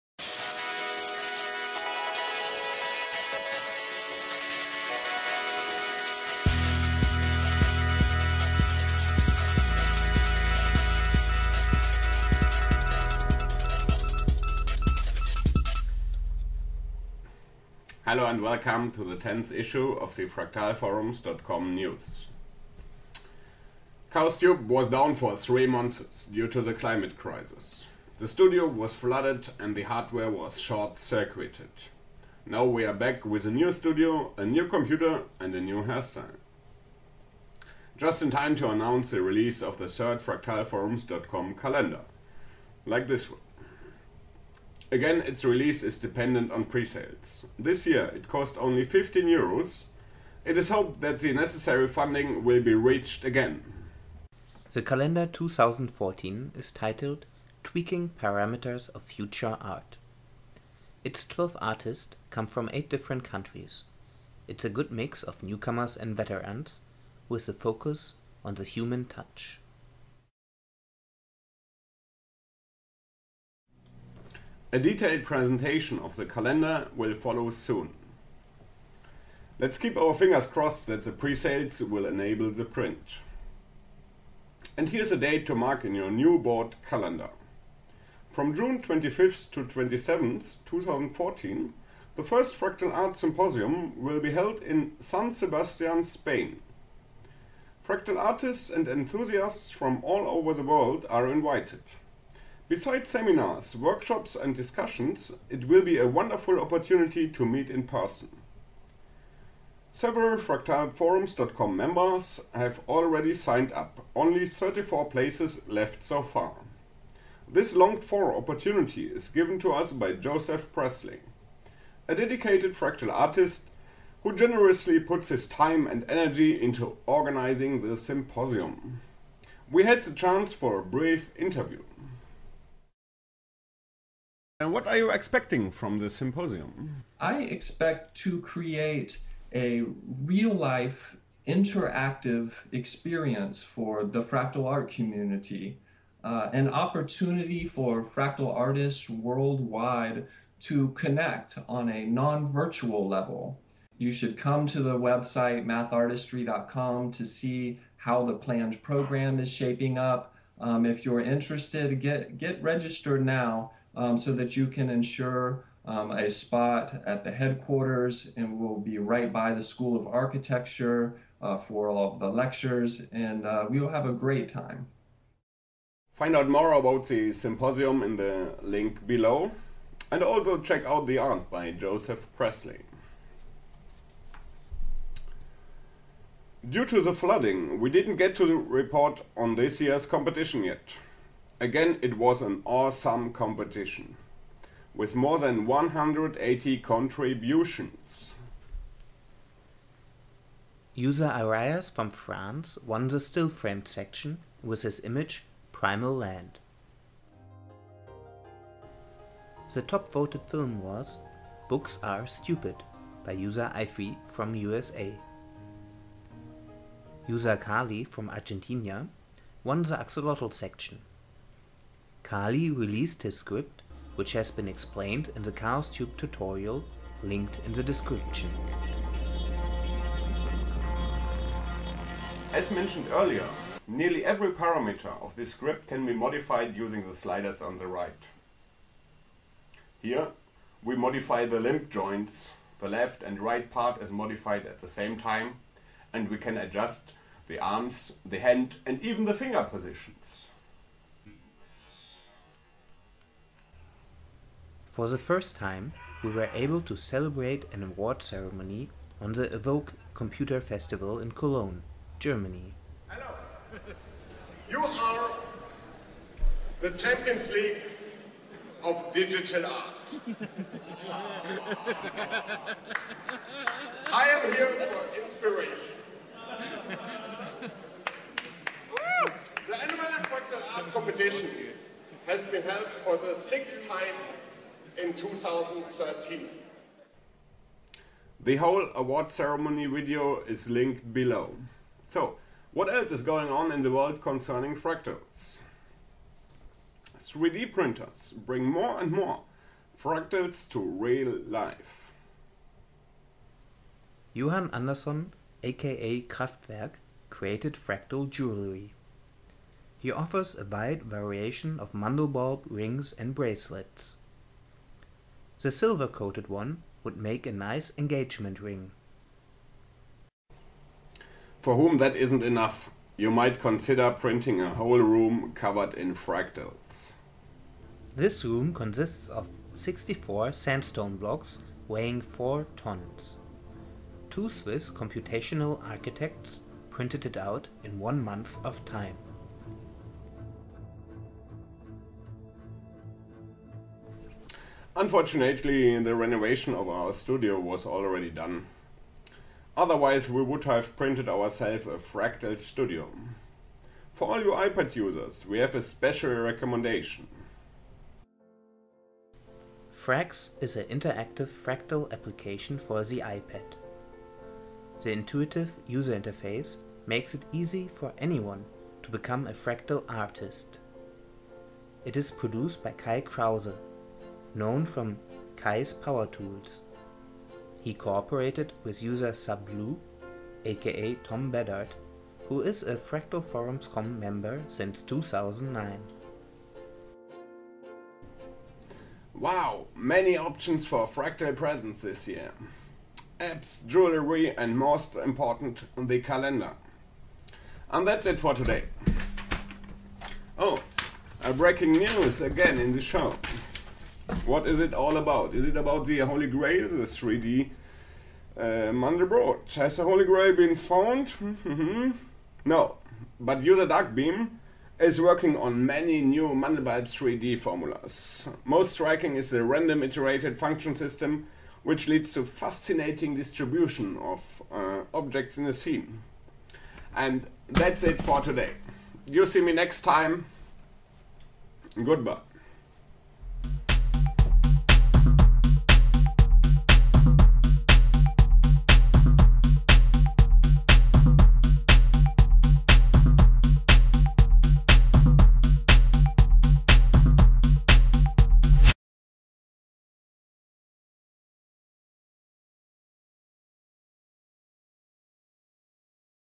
chaosTube news #10 - Fractal Calendar - Fractal Symposium - 3d Printing - Frax
Since 2021 I Love Chaos took over, Chaos Theory, Art and Philosophy. Don't discredit Chaos.

ChaosTube News #10

- Fractalforums.com Calendar Presales:
chaosTube news #10 - Fractalforums.com Calendar #3 2014 - Fractal Art Symposium - 3d Printing - Frax

fractalforums.com is a thriving web-forum of enthusiastic fractal fanatics. beside finding the mandelbulb and the now famous mandelbox it is an active platform with new ideas worth spreading